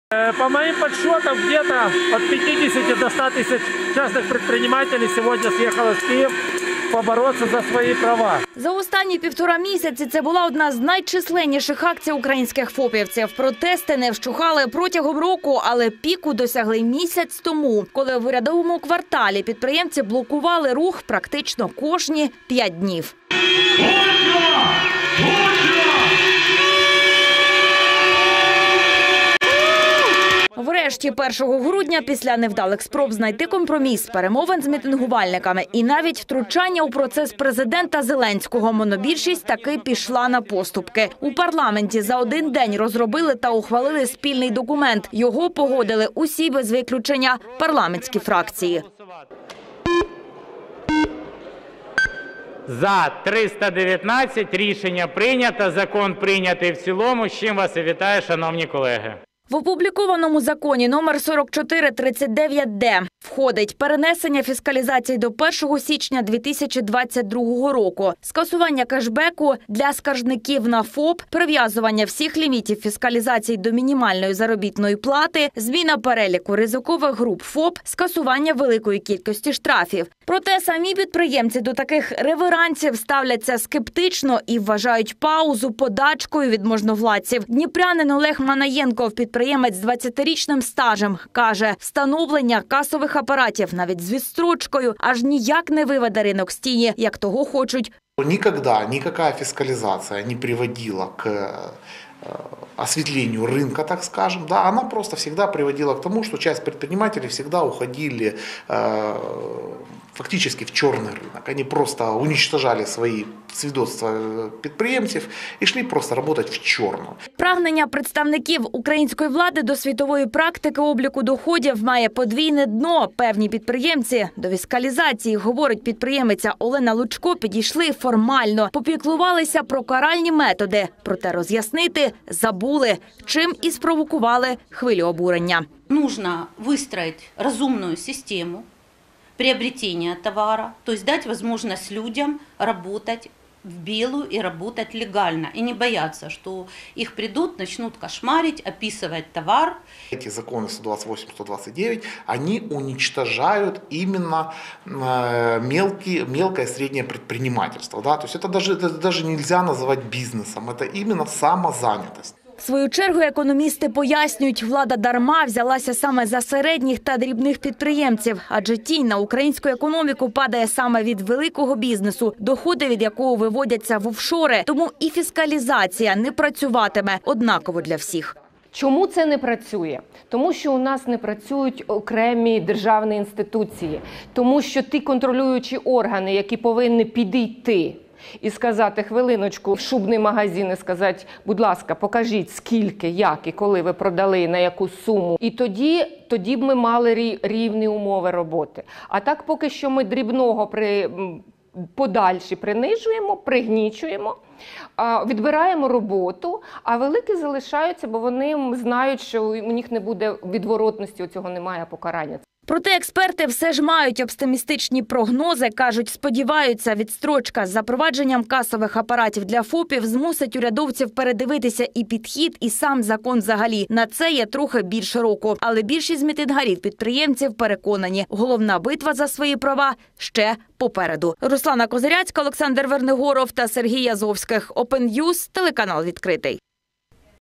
Бізнес занепаде, але трохи пізніше - так кажуть підприємці про дворічну відстрочку касових апаратів
Українські парламентарі відклали фіскалізацію малого та середнього бізнесу до 1 січня 2022 року. За компромісний проєкт закону, який відкладає запровадження касових апаратів для більшості категорій ФОПів, 1 грудня проголосували 319 народних обранців. Зробили це під тиском численних протестів. Підприємці понад місяць мітингували в центрі столиці. Однак великих надій на цю відстрочку не покладають. Кажуть – рано чи пізно, ця новація знищить їхній бізнес, якщо влада не розпочне діалог._______________________________________________